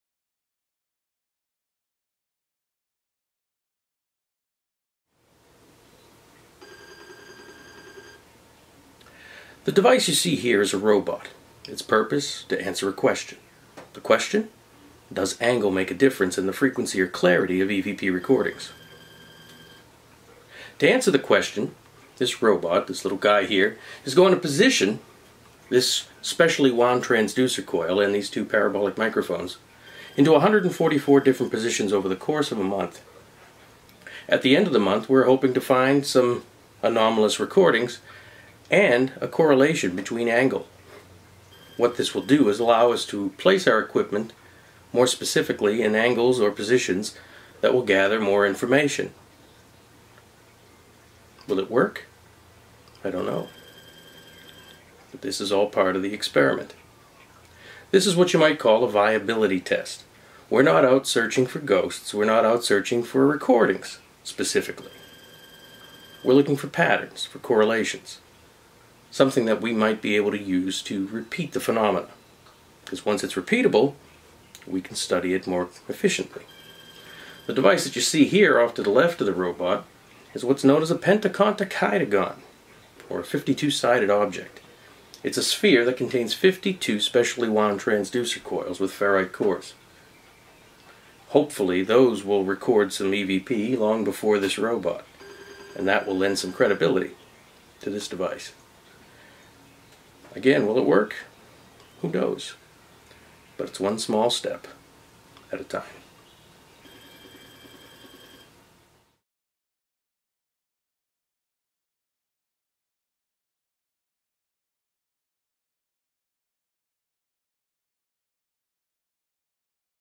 Paranormal Robot
This is a very brief demonstration of a robot that I created to assist with viability testing of an angle hypothesis. The idea is to test whether angle and position affect the frequency and/or clarity of EVP recording. The goal is not specifically to record EVP phenomena, but rather to correlate the angle of the device with the recording of anomalous audio. This robot uses a specially made (and highly sensitive) transducer and two parabolic microphones. It also records air quality, barometric pressure, humidity, temperature and a variety of gas levels. The transducer output feeds into an auto analyzer that will look for anomalous activity and store the "hits" in a separate folder for later review.  The files will ALL be analyzed using spectrograph technology and phonetic analysis will be performed on all audio.

Michael J. Baker